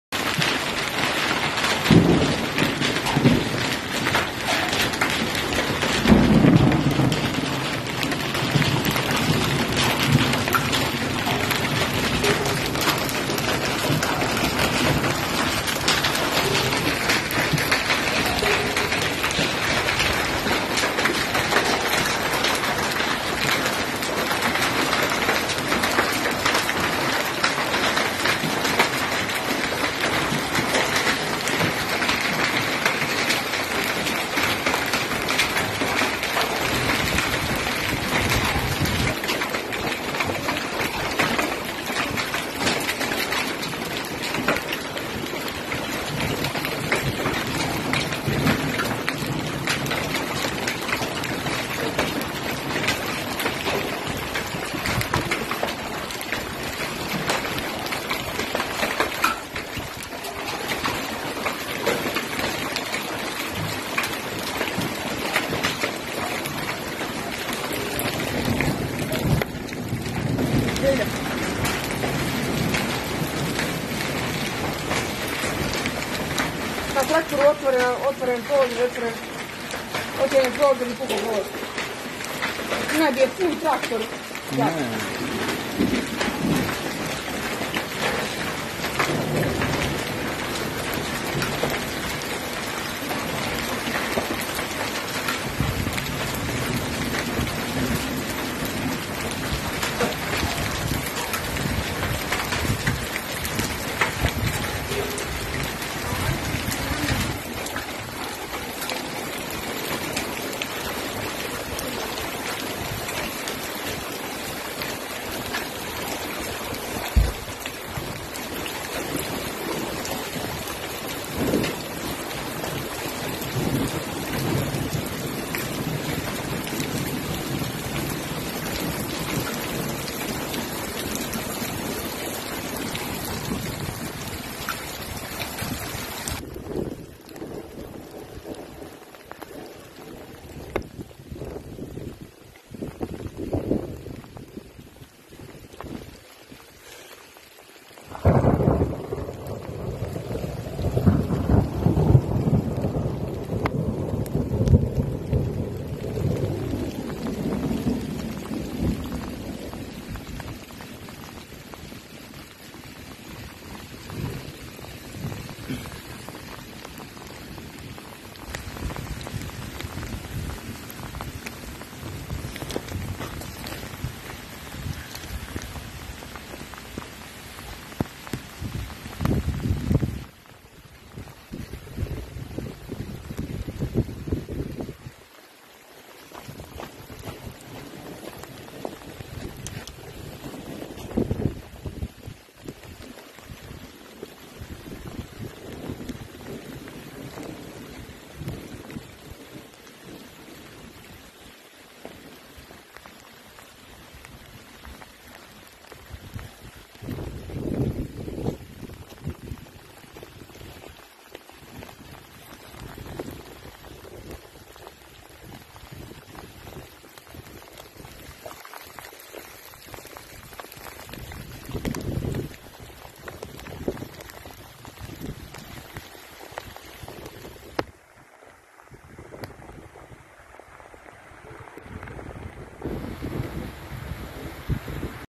Grad veličine oraha obeleo nekoliko sela u Ivanjici
Grad ni ovog puta nije zabišao okolinu Ivanjice. Padao je nekoliko minuta ali dosta krupan, u pojedinim zaseocima čak veličine oraha. Da li je prouzrokovao veće štete u malinjacima i na drugom voću znaće se tek sutra. Dragana Perišić kaže da je sva sreća što je kratko trajalo jer bi sigurno posledice bile katastrofalne.

-Već je uhvatio mrak pa nismo mogli da uđemo u malinjake ali ujutru ćemo videti kolika je šteta. Sigurno je lomilo grane jer je bilo i grada i oluje. Šta da radimo, nije prvi put, da smo zdravo samo.-kaže Dragana.

Grad je najviše pogodio sela Deretin, Međurečje, Kušiće, Bratljevo, Kumanicu, Komadine...